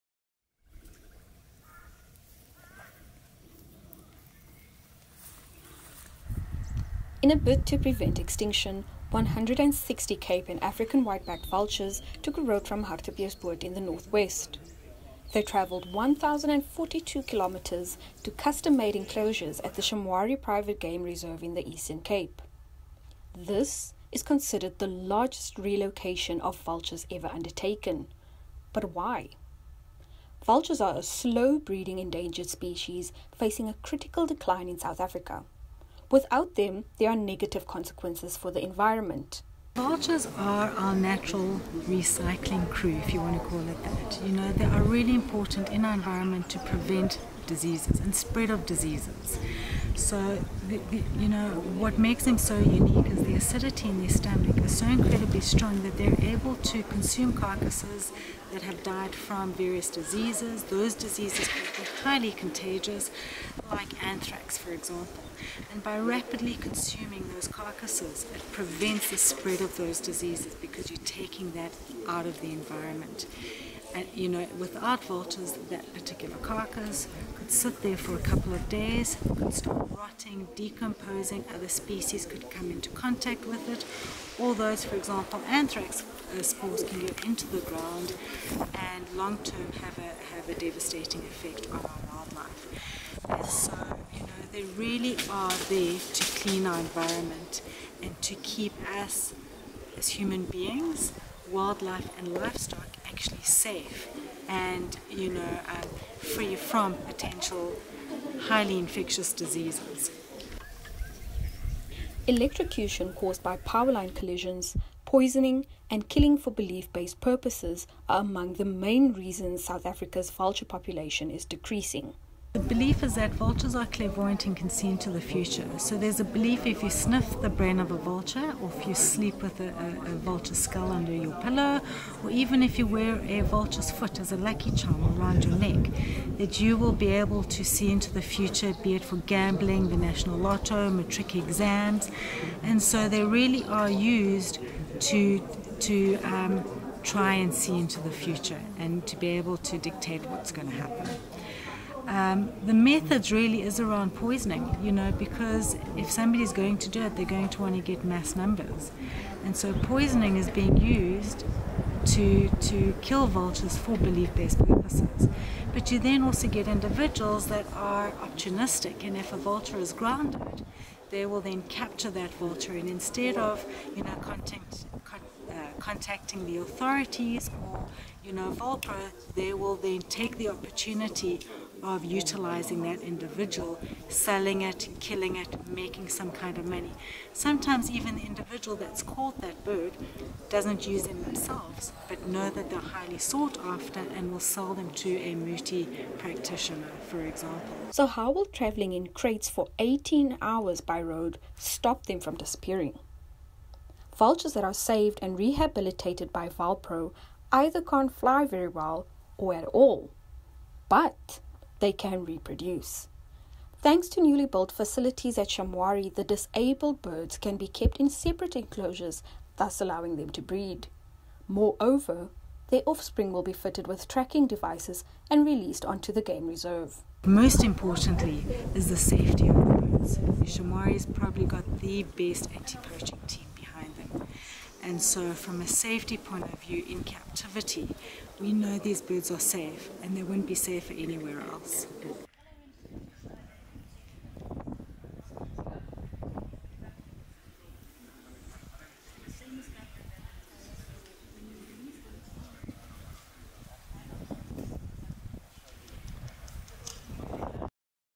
Why the historic move of 160 vultures to the Eastern Cape is good for the environment
In a bid to prevent extinction, 160 Cape and African white-backed vultures took a road trip from Hartebeespoort in the North West. They traveled 1,042km to custom-made enclosures at the Shamwari Private Game Reserve in the Eastern Cape. EWN was there for their arrival to find out why this relocation was so important.